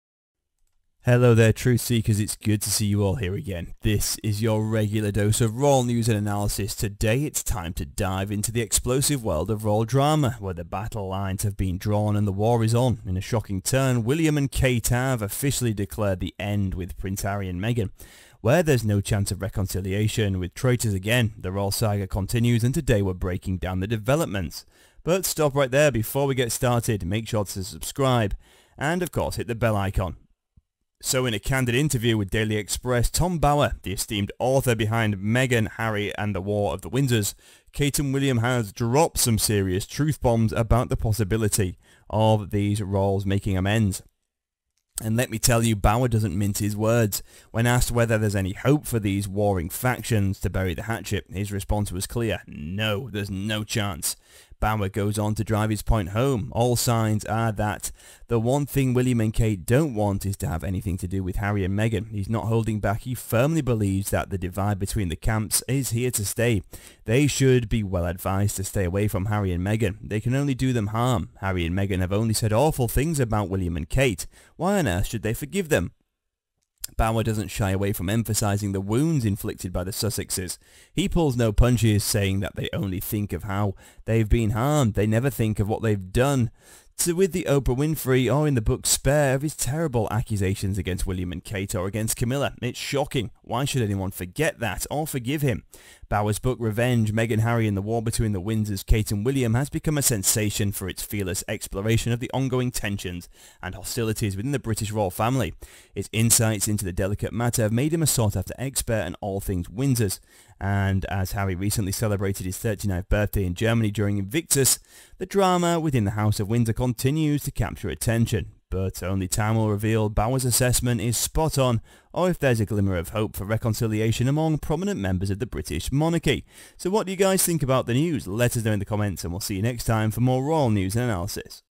IT'S THE END FOR YOU! Furious William & Kate DESTROY Evil Meghan Markle and Harry to Protect Family.
So, In a candid interview with the Daily Express, Tom Bower, the esteemed author behind "Revenge: Meghan, Harry and the War Between the Windsors, Kate and William," has dropped some serious truth bombs about the possibility of these feuding royals making amends. And let me tell you, Bower doesn't mince words. When asked whether there's any hope for these warring factions to bury the hatchet, his response was crystal clear: "Absolutely not. No, there's no chance of any reconciliation."